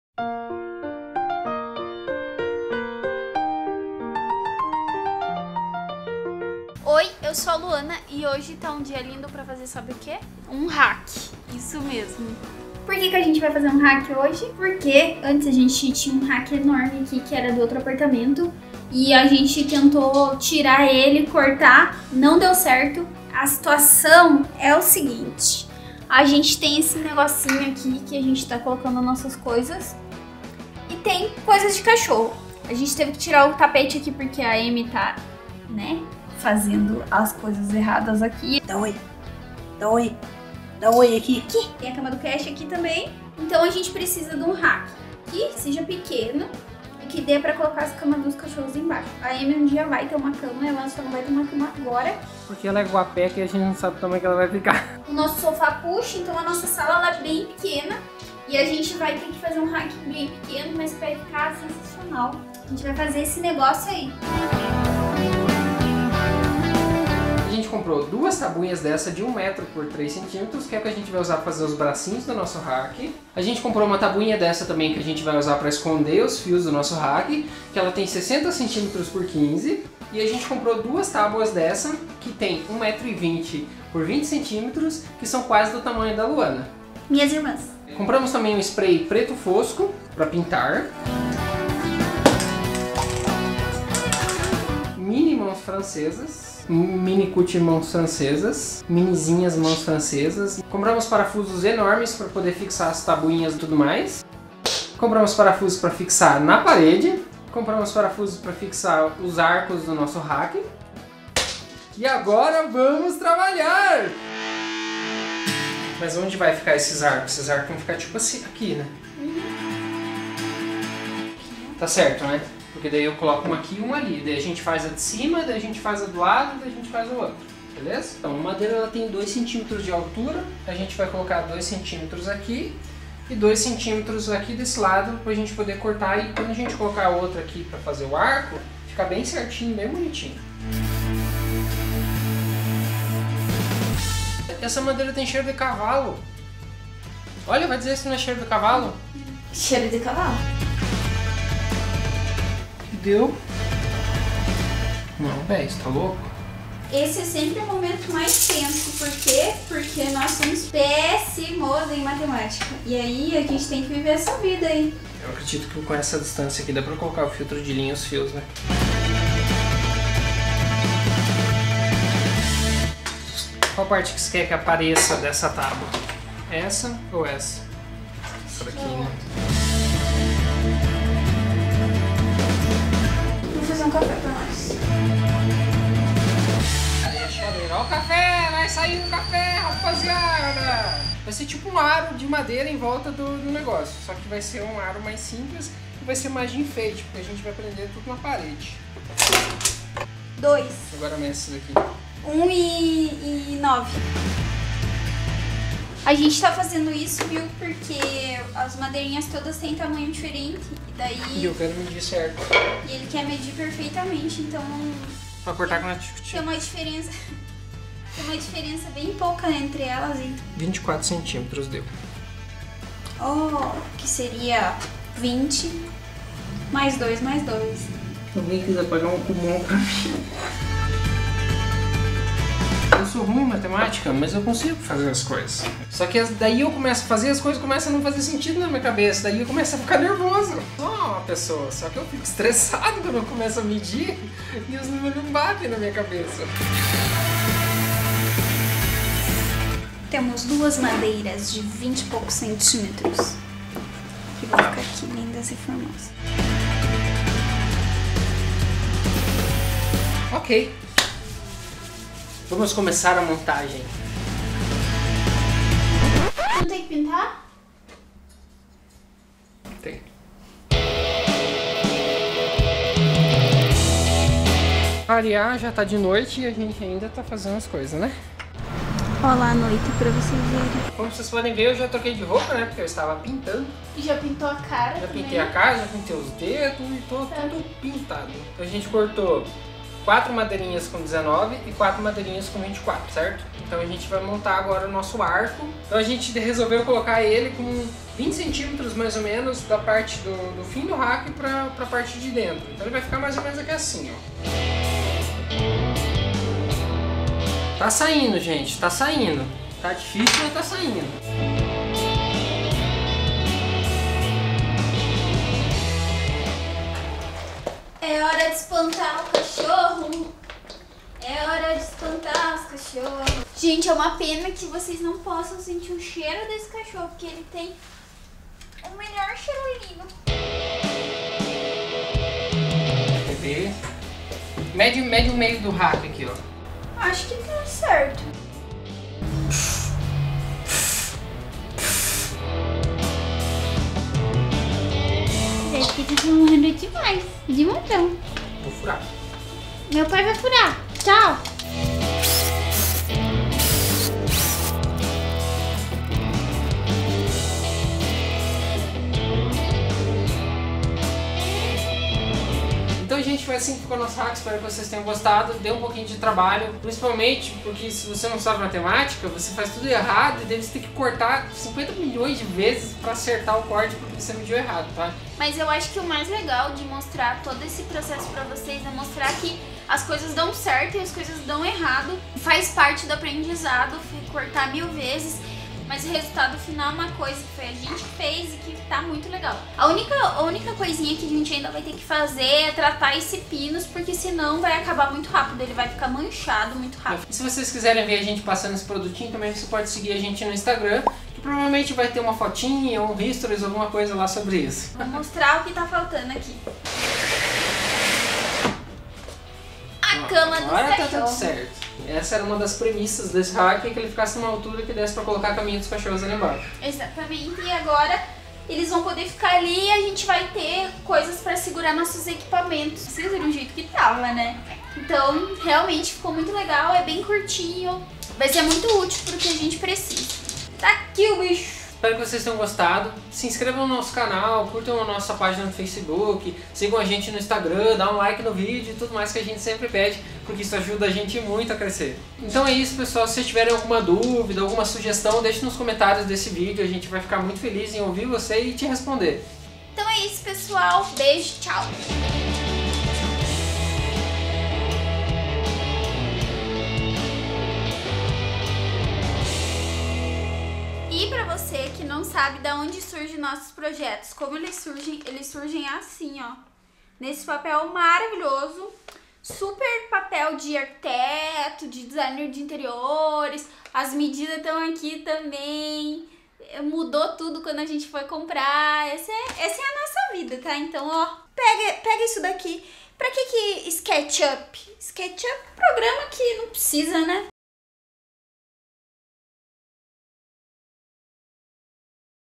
COMO FAZER UM RACK DE TV DE MADEIRA PINUS | DIY | DE APÊ NOVO
Nesse vídeo mostramos como fizemos nosso rack de madeira pinus.
Sim, estávamos muito cansados nesse dia, mas não se abalem, estamos bem! s2

Ferramentas:
Furadeira impacto

- Materiais:
2 tábuas de eucalipto de 1metro x 3 cm ( São ripas, mais grossinhas);
1 tábua de pinus 60cm x 15 cm;
2 tábuas de pinus 1.20cm x 20cm;
1 spray preto fosco
2 “mini” mãos francesas
4 parafusos grandes para fixar as tábuas
6 parafusos com bucha para fixar o móvel na parede.
8 parafusos menores para fixar os arcos
Vídeozinho com o passo-a-passo.

Seja membro e ajude esse canal a continuar a existir, com o membros você tem acesso a videos de making of, e participa da decisão dos conteúdos  :)

Quer mandar algo pra gente?